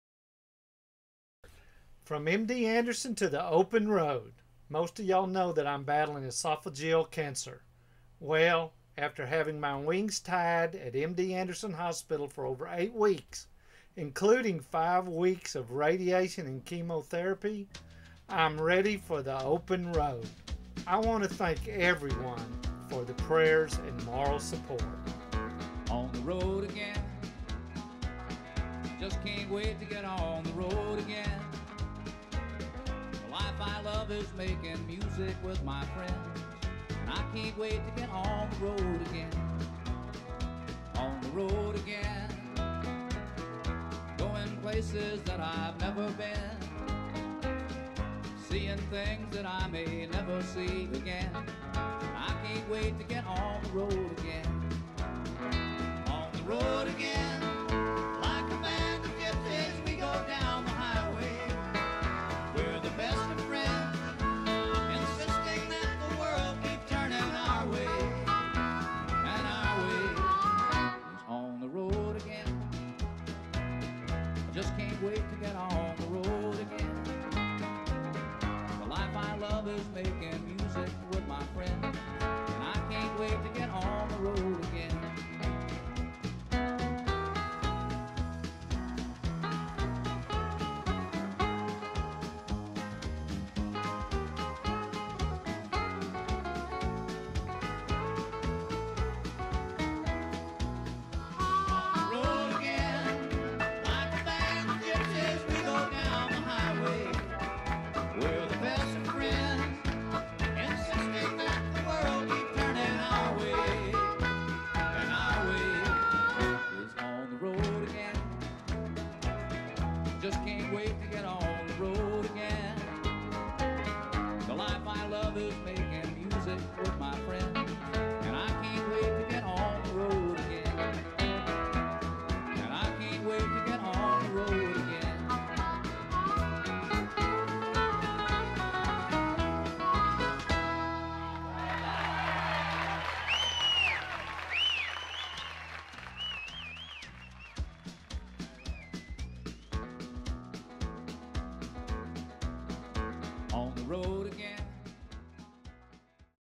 Craig Dickson is On The Road Again
Chasing the Texas Sunsets and On the Road Again.